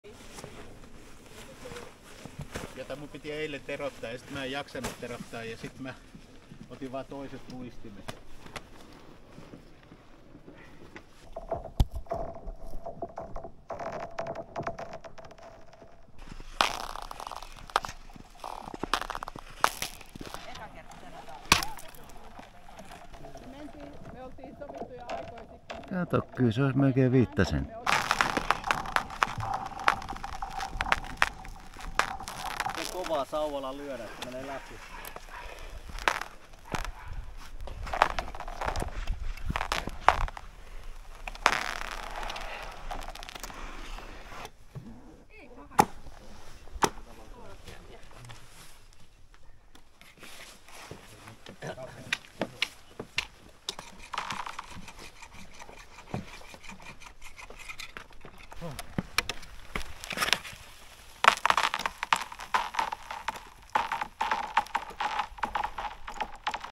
2023-11-19 Kauden avaus nuuksiossa
Ohuella jäällä luistelua. Paksuus 37-45mm